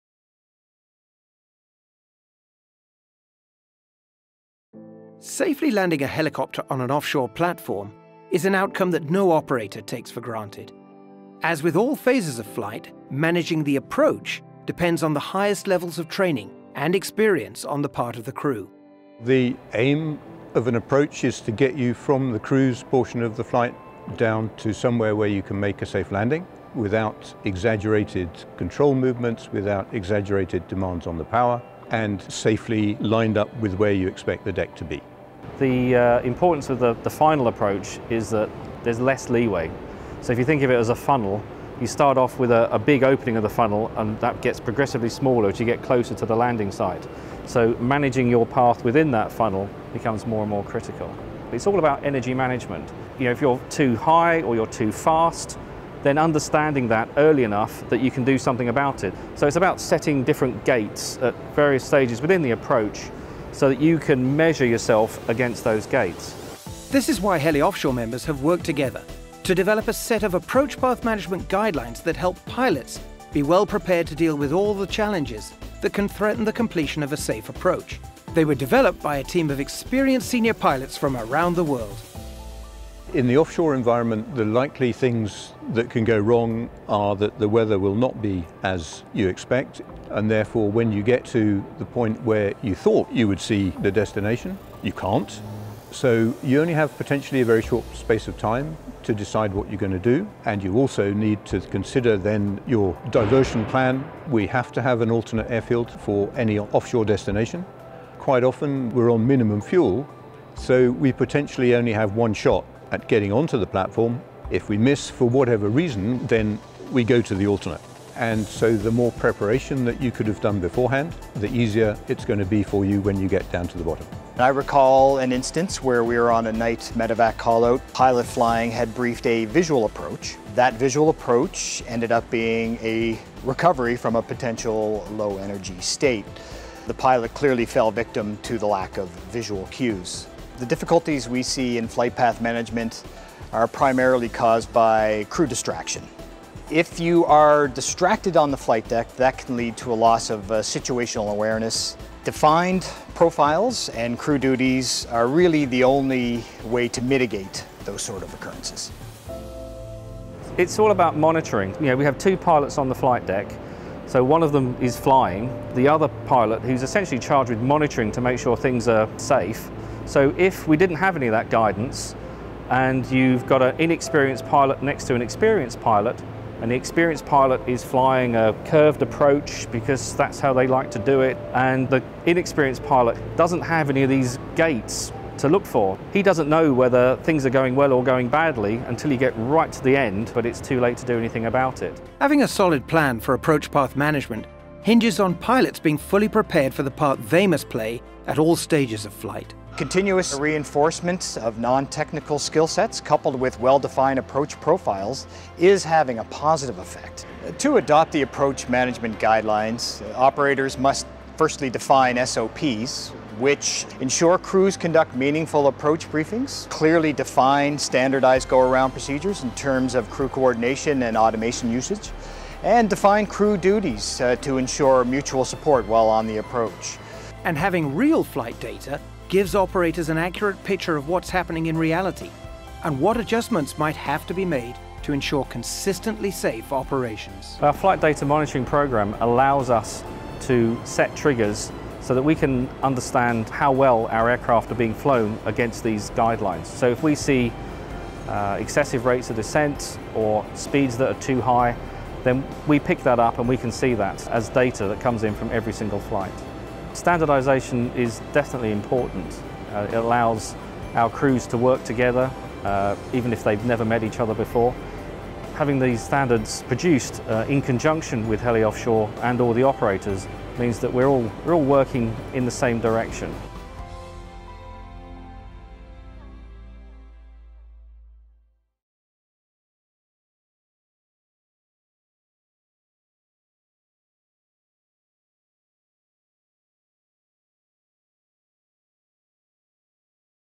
How Approach Path Management Supports Safe Landings on Offshore Platforms
The right training and preparation are critical to helping offshore helicopter pilots to safely manage the approach path phase of flights. In this video, senior pilots and safety experts from HeliOffshore member companies explain how this can be achieved.